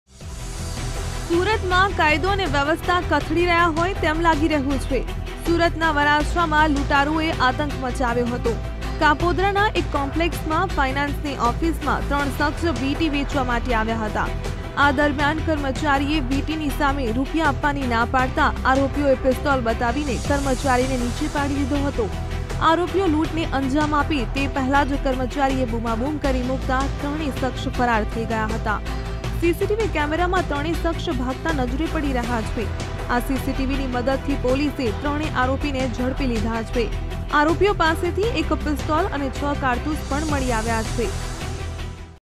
Surat માં 3 શખ્સે કર્યો લૂંટનો પ્રયાસ, કર્મચારીએ બૂમાબૂમ કરતા આરોપી રફૂચક્કર
Surat માં 3 શખ્સે કર્યો લૂંટનો પ્રયાસ, કર્મચારીએ બૂમાબૂમ કરતા આરોપી રફૂચક્કર | VTV GUJARATI